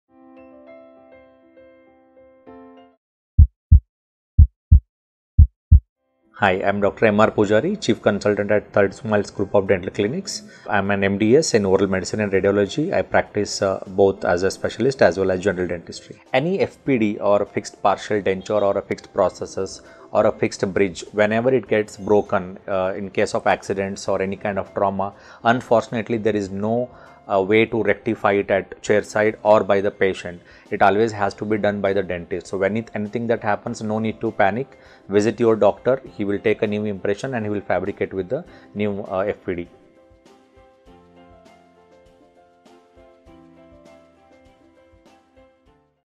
Tooth Crown breakage after traumatic dental injury | FPD Breakage - Dr. Pujari M R| Doctors' Circle
Dr. Pujari MR | Appointment booking No : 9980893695
Consultant Dental Surgeon | 32 Smiles Multispeciality Dental Clinic, Kundalahalli,Bangalore
Any  FPD or fixed  partial denture or  fixed prosthesis,  or  a  fixed  bridge, whenever it gets broken in case of  accidents or  trauma, there is no way to  rectify  at chair side  or  by the patient. It  always   has to be done  by the dentist. So if  anything happens, no need to  panic, you can visits  or  doctor and he can take  an  impression and  will fabricate  with a  new  FPD.